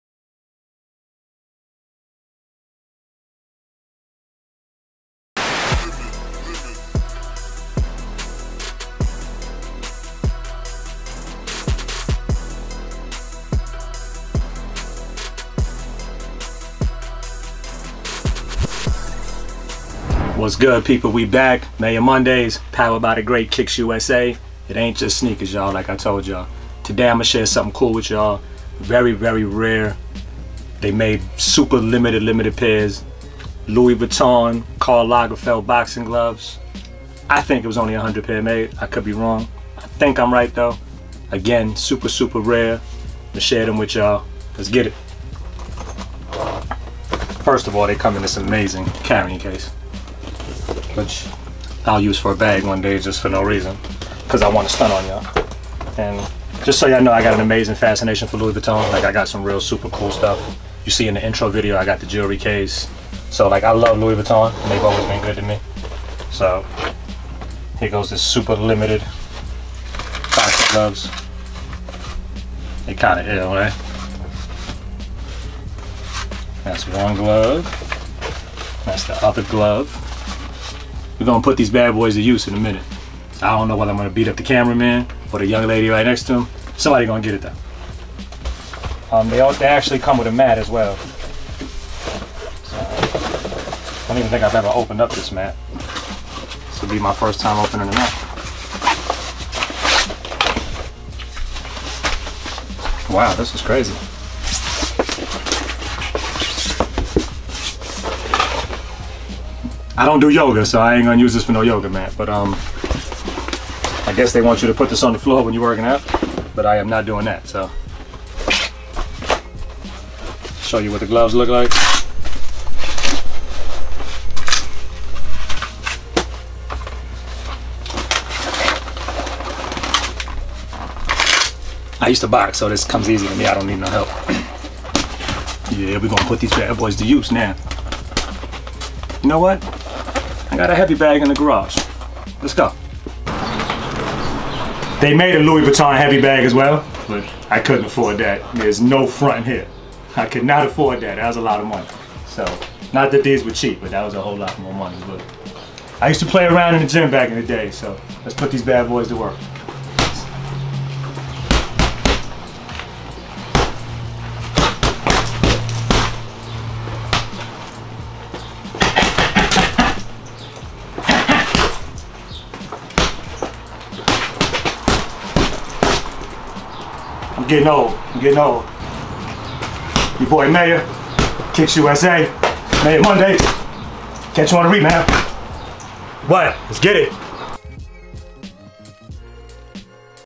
Mayor Mondays | Louis Vuitton Boxing Gloves
Welcome to Mayor Mondays, where every week Mayor shows us something rare and exclusive from his closet. This week he is giving us an exclusive look at his exclusive Louis Vuitton Boxing Gloves that have never been used before.